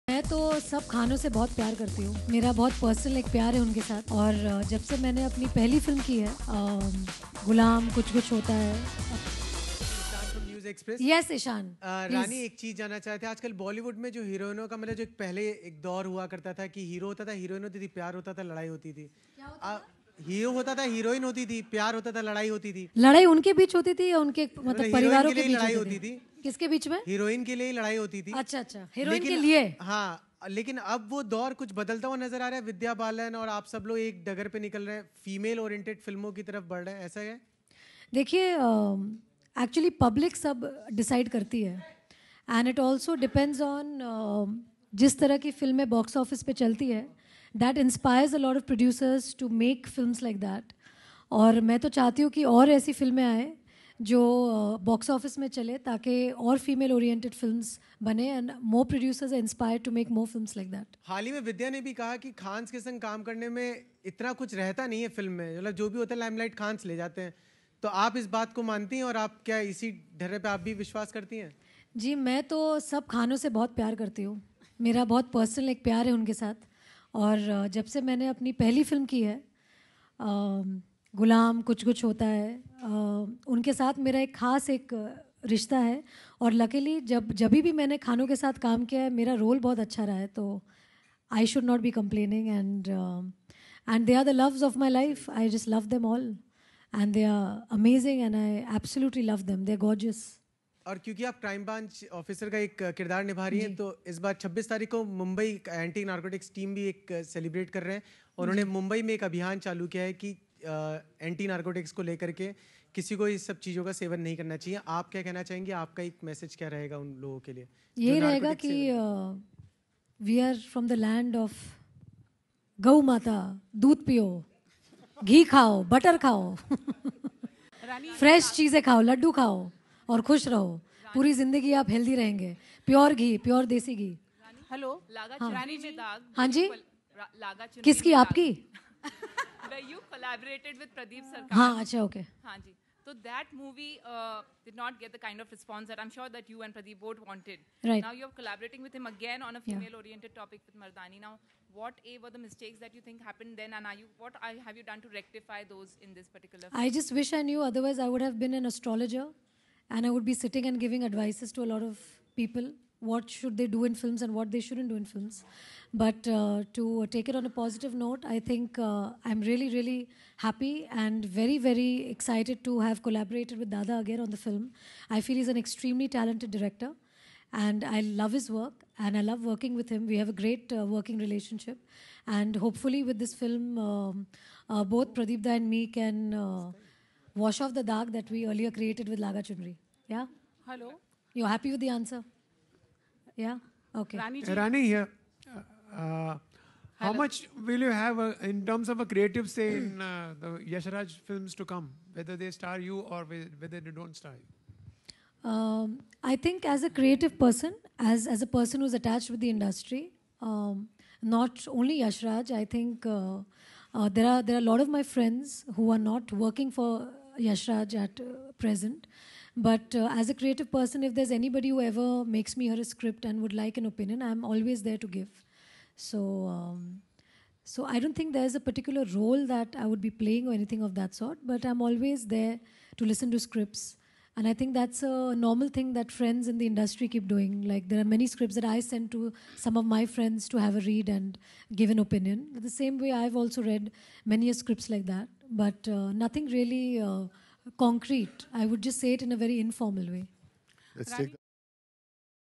Rani Mukherji At Trailer Launch Of Movie Mardaani Part 6
Rani Mukerji spoke to the media after a long hiatus at the trailer launch of her upcoming film Mardaani. Rani will be seen in a Dabangg avatar in this one as she will eb playing a cop who busts a big child trafficking racket

Rani told the media that she will continue doing film and she wants to be a homemaker now. She also said that Aditya Chopra will be looking after the Yash Raj Films front and that she doesn't intend to participate in the production business.

About Preity Zinta's case Rani said that she was surprised to know that Preity was having a tough time as she has always known  her Chori Chori Chupke Chupke co-star to be a tough nut.

We hope that Mardaani marks the newly-wed actor's comeback and turns her career around!